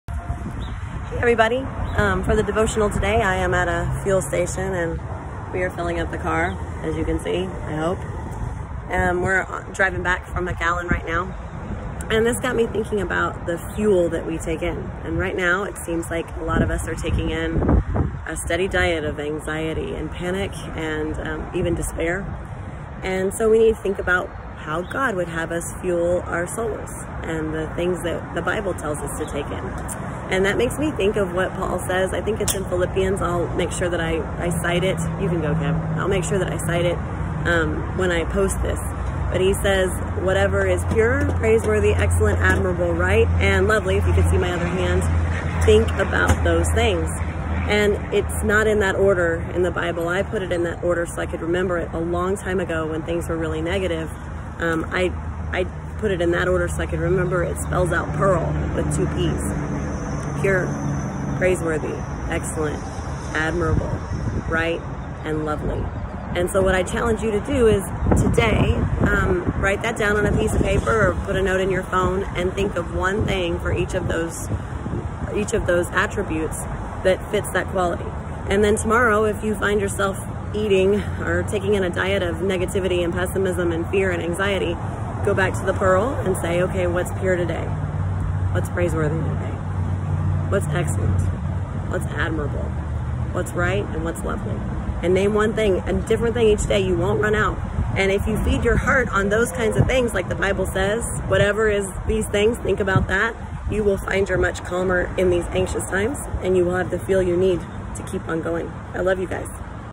Fuel
Let’s stop fueling up on panic. There's a better way to fuel our spirits in hard times. Pop-up Devotional from March 17, 2020 as Texas was going into lockdown for COVID 19.
“And now, dear brothers and sisters, one final thing. Fix your thoughts on what is true, and honorable, and right, and pure, and lovely, and admirable. Think about things that are excellent and worthy of praise.”
‭‭Philippians‬ ‭4:8‬ ‭